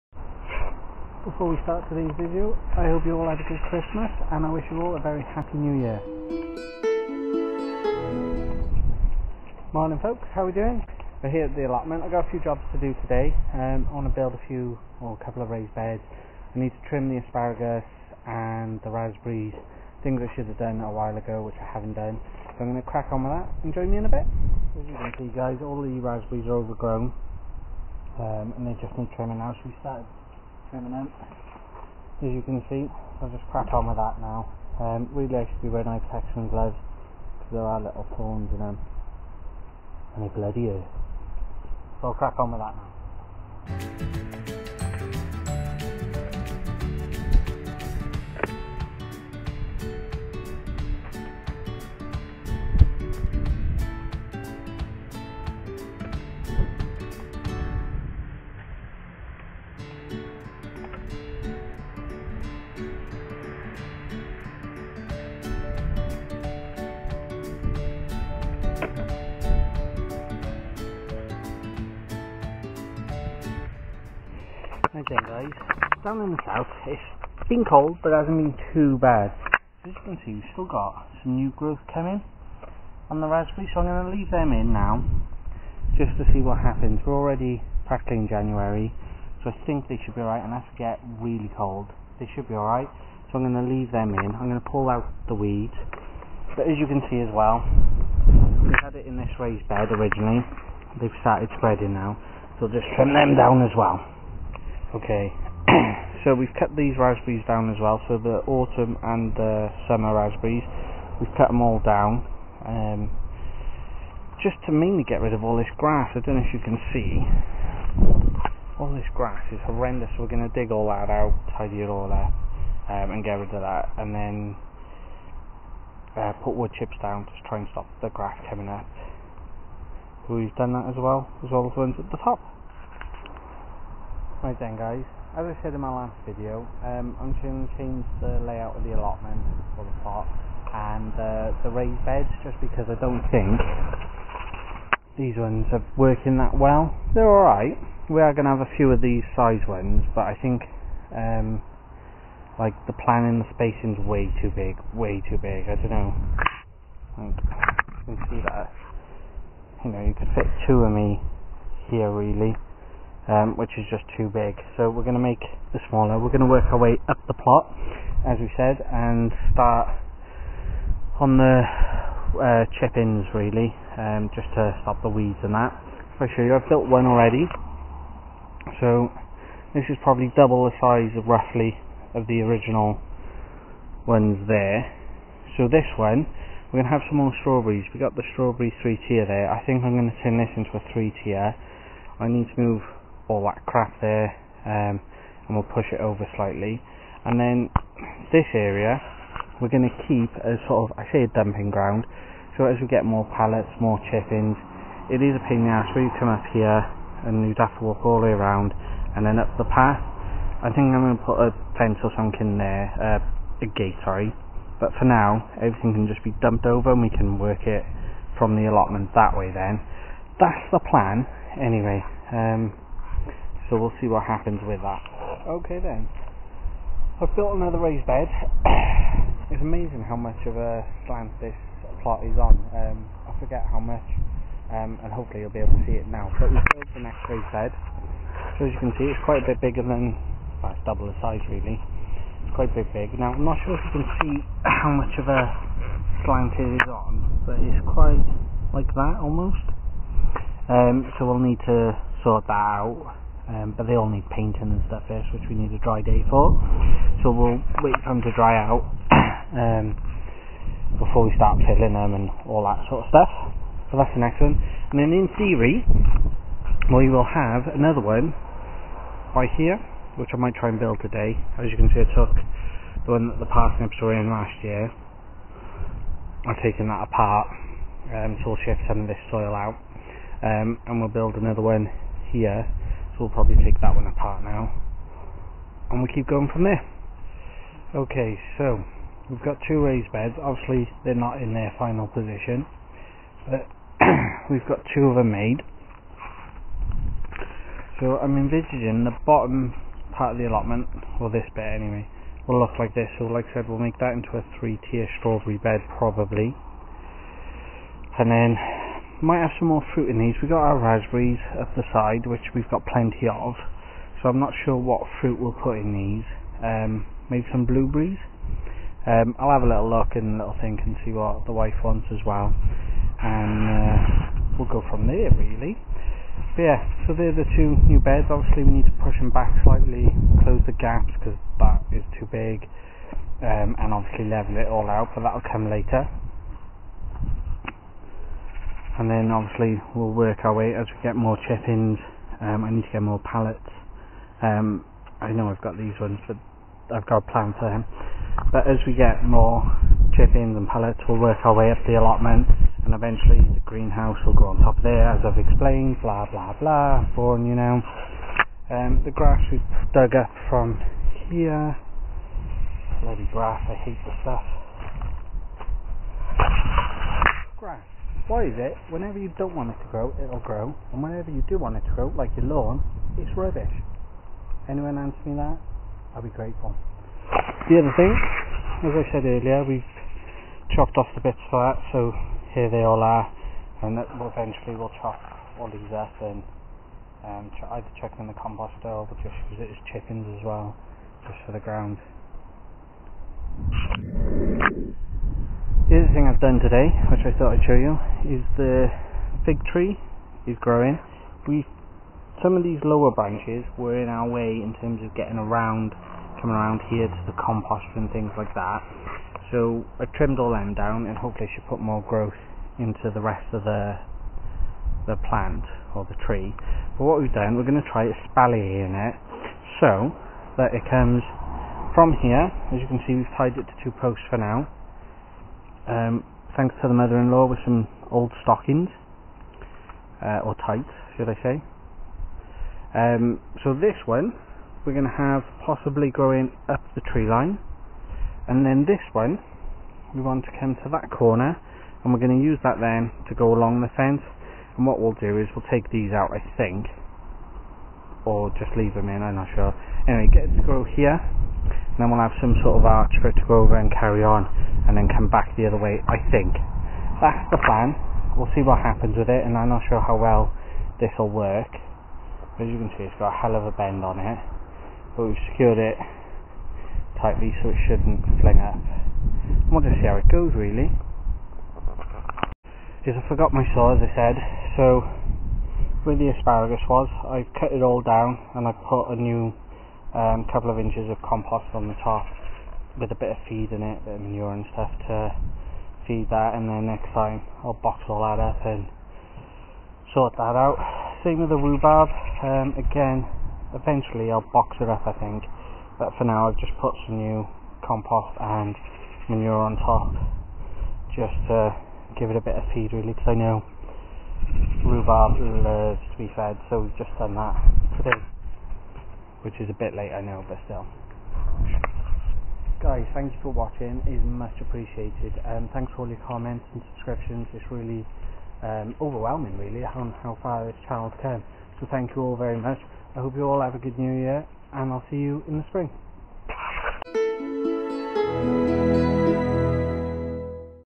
My Allotment Diary UK, plot update from December. New plan and raised bed
Hi guys happy new year to you all.

This video is an update from december and where I am at.

I have a new plan of action to hopefully kick start the allotment for the future.

Thanks

Nad

Join me on my journey rat the allotment to grow my own fruit and Veg "My Allotment Diary, in UK - We have a delivery at the plot. Plus April plot tour 2021"